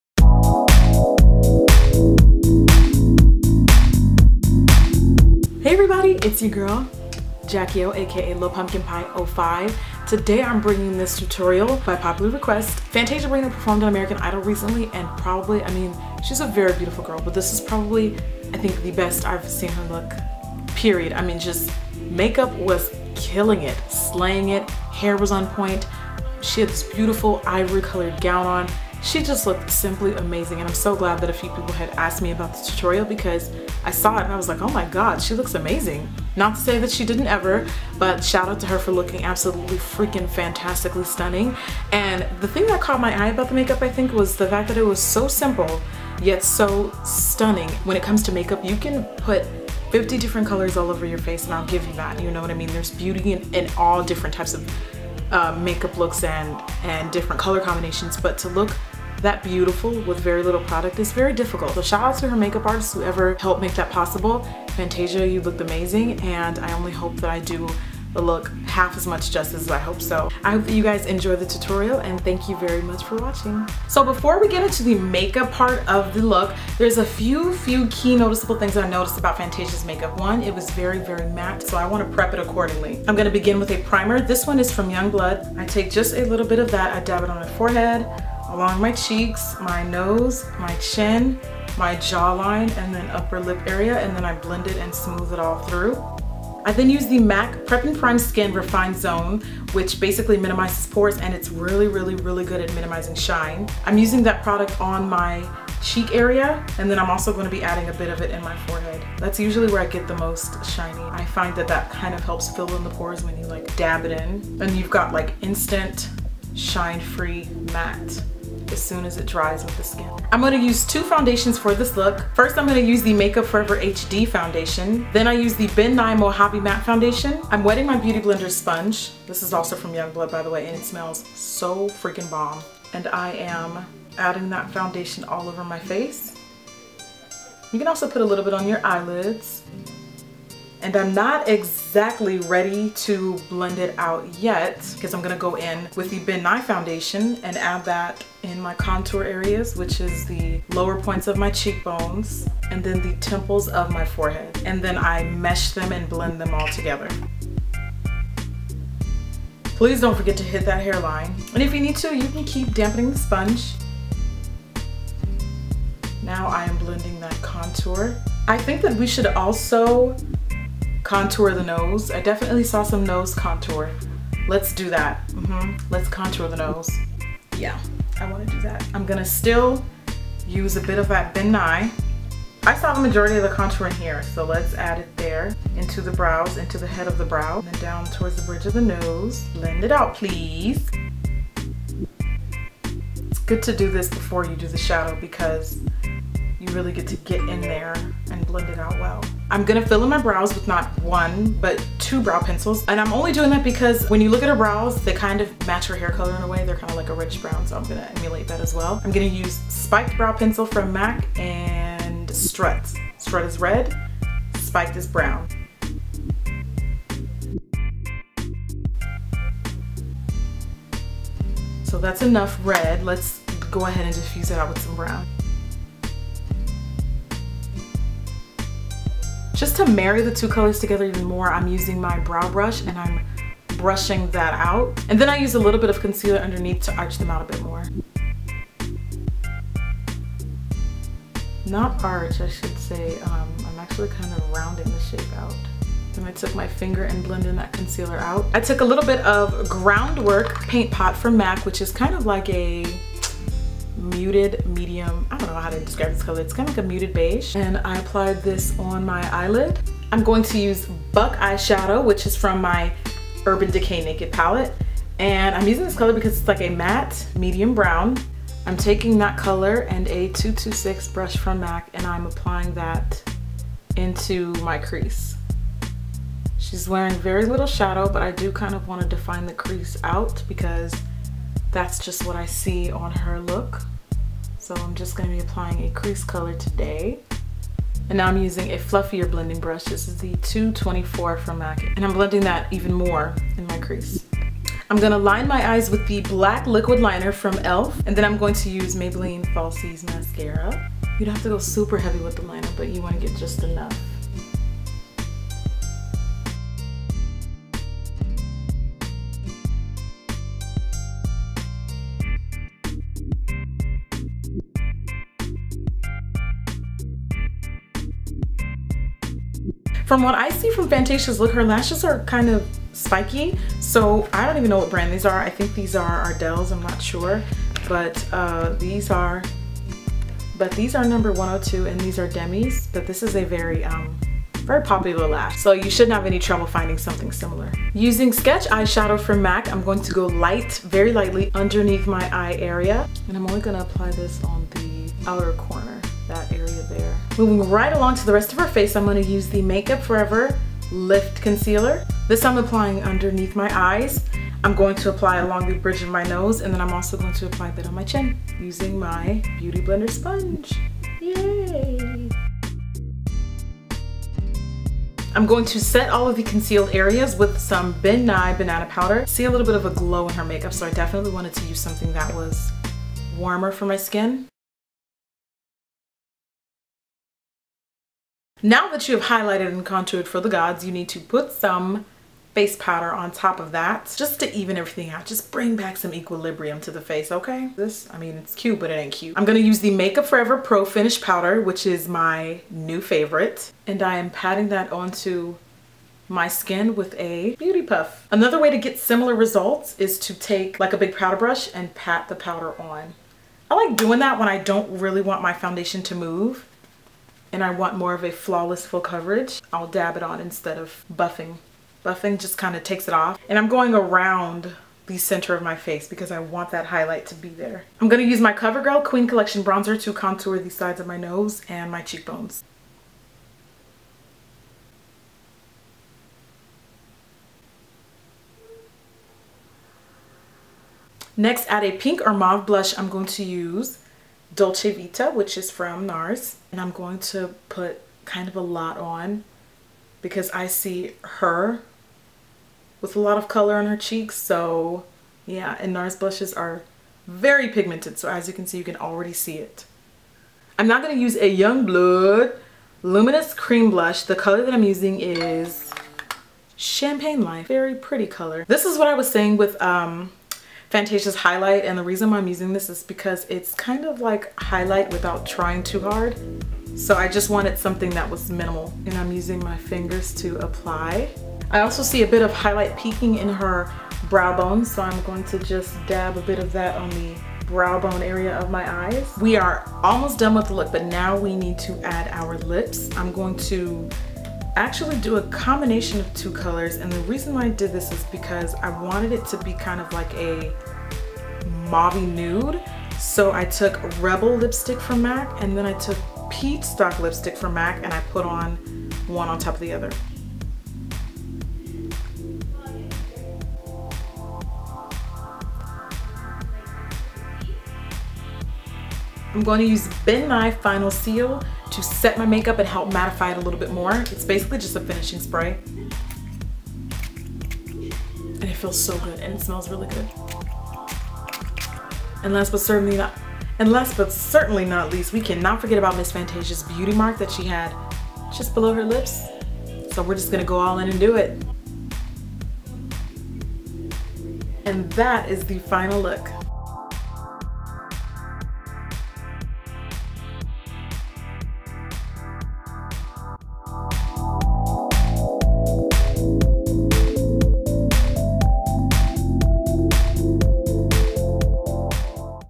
Fantasia Barrino on American Idol Makeup Tutorial
A makeup tutorial inspired by Fantasia Barrino on an episode of American Idol!

Keep it connected!